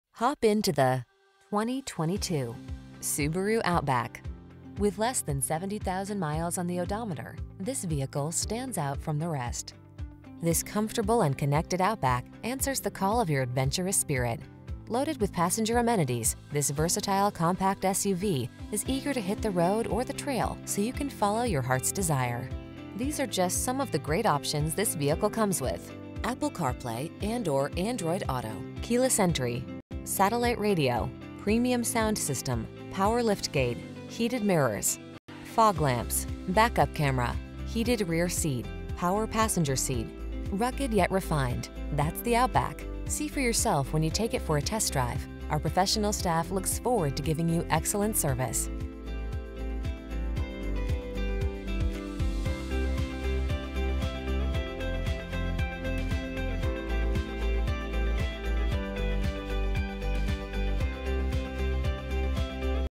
2022 Subaru Outback at Oxmoor Toyota Louisville & Lexington, KY U28904
Crystal Black Silica Used 2022 Subaru Outback Limited available at Oxmoor Toyota in Louisville, Kentucky. Servicing the Lexington, Elizabethtown, KY New Albany, IN Jeffersonville, IN area.

Oxmoor Toyota
8003 Shelbyville Rd